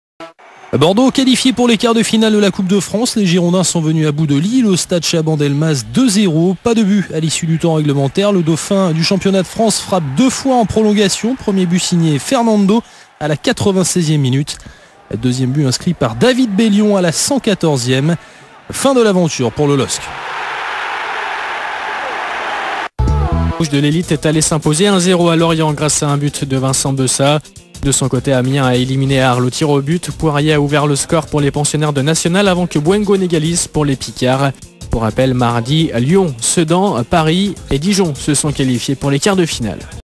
Bordeaux 2 - 0 Lille (19-03-2008) Coupe de France
Stade Chaban-Delmas - 19 Mars 2008
Coupe de France (1/8 finale)
Buts : F. Menegazzo 96’ et D. Bellion 114’.
Avertissements : U. Ramé (J, 111), S. Lichtsteiner (J, 111), P. Franquart (112).
Bordeaux : U. Ramé ; D. Jemmali (F. Jurietti, 81è), C. Henrique, S. Diawara, F. Marange ; A. Diarra, A. Alonso (F. Menegazzo, 46è), J. Micoud (D.Bellion, 69è), B. Trémoulinas ; M. Chamakh, F. Cavenaghi.
Lille : T. Sylva ; F. Béria, A. Rami, P. Franquart, G. Tafforeau ; M. Debuchy (S. Lichtsteiner, 75è), R. Mavuba, J. Makoun ; P.A. Frau (N. Fauvergue, 81è), C. Makiese (L. Obraniak, 62è), F. Michel Bastos.